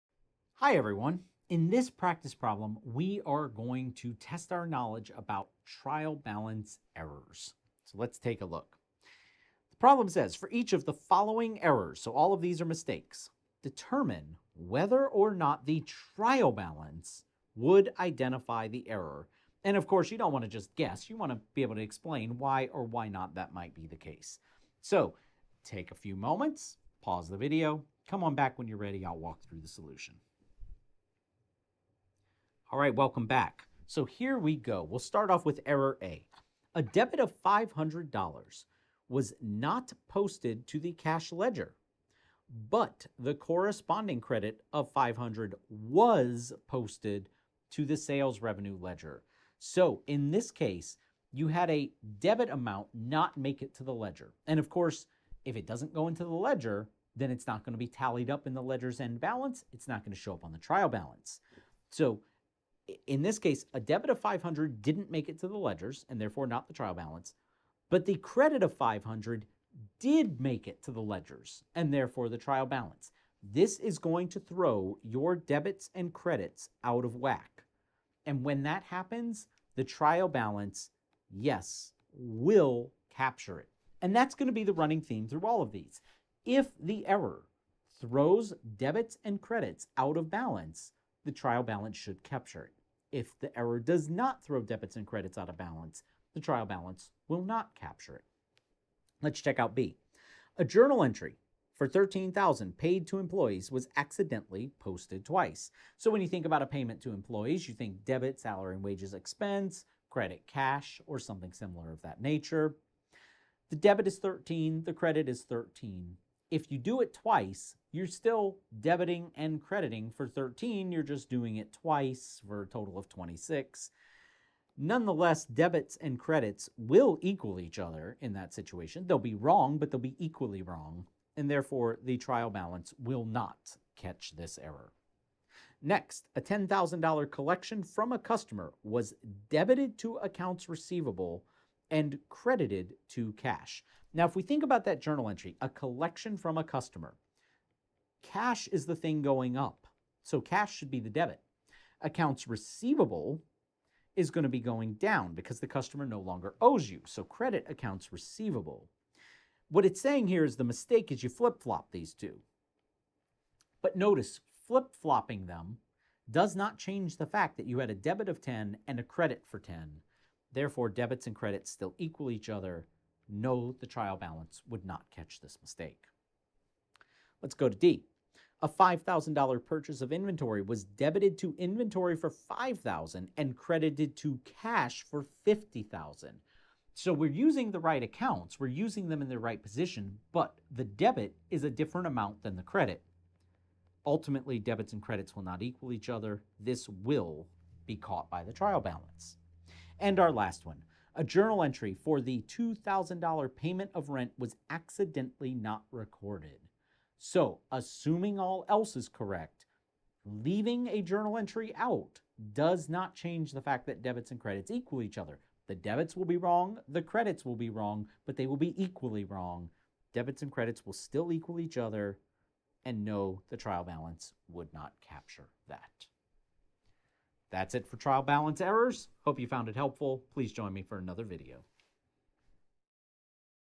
Practice Problem TB-03: Trial Balance Errors
This video contains a practice problem requiring students to determine whether a set of accounting errors would be caught by preparing the unadjusted trial balance.